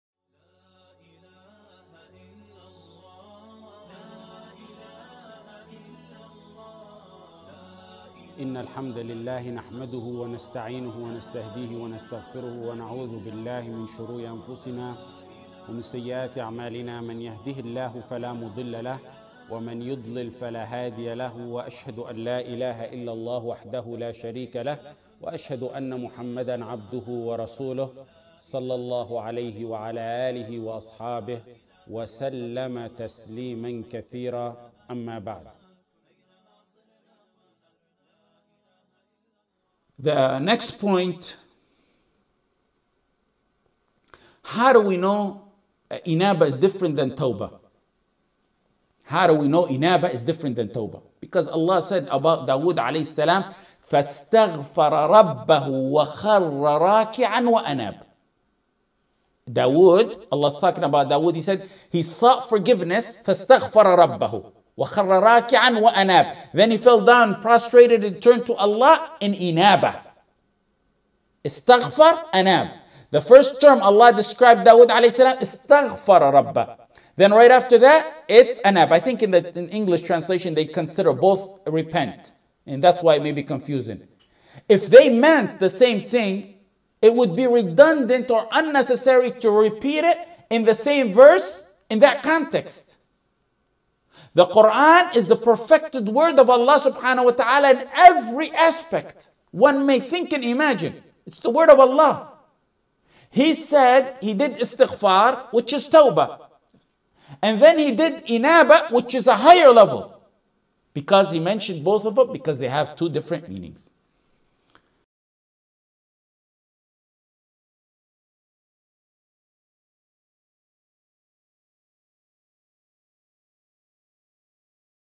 How Do We Know Inaabah Is Different To Tawbah? | Shaykh Ahmad Musā Jibrīl (حفظه الله)
Allah said about Dawood ‘alayhis-salaam:
And he sought Forgiveness of his Lord, and he fell down prostrate and turned (to Allah) in repentance. (Surat Saad: 24)